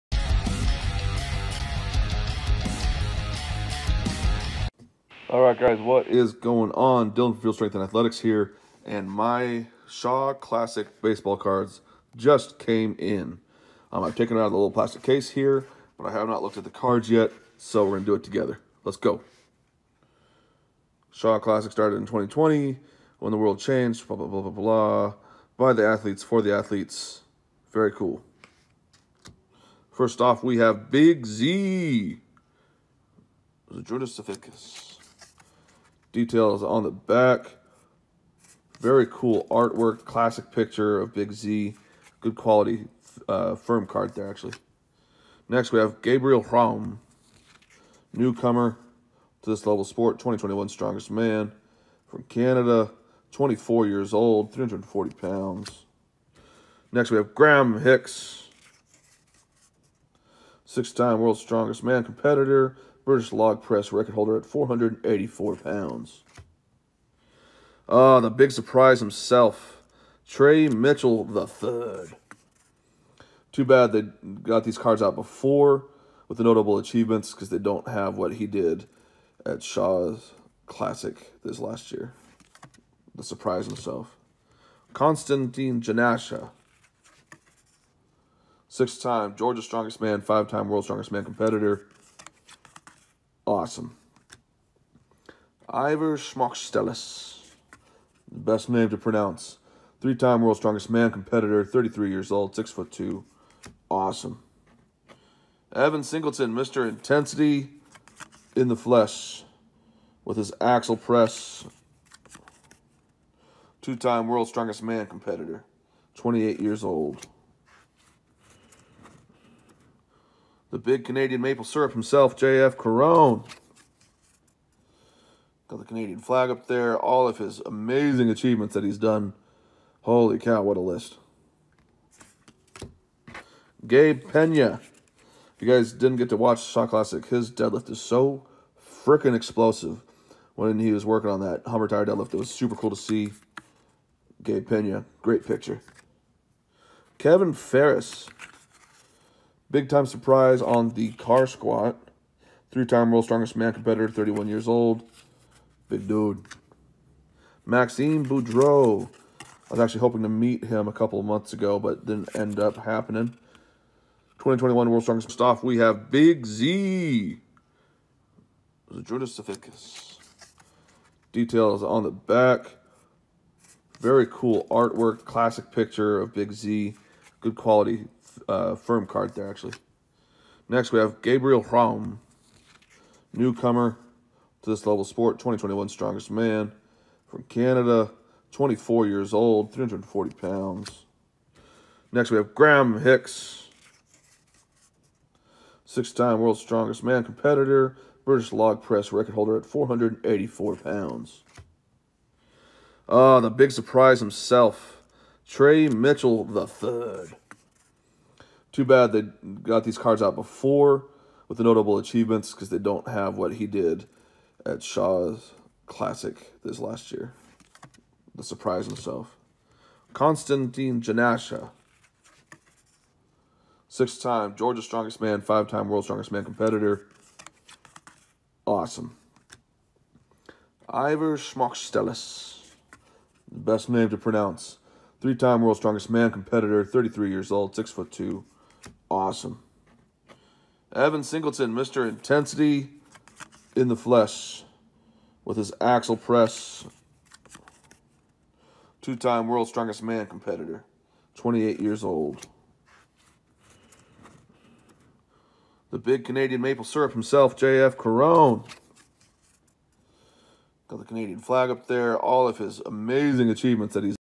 SHAW CLASSIC "BASEBALL CARDS"
They have finally come in!  My very own Shaw Classic "Baseball Cards."  With one card for every athlete that competed in the 2021 Shaw Classic.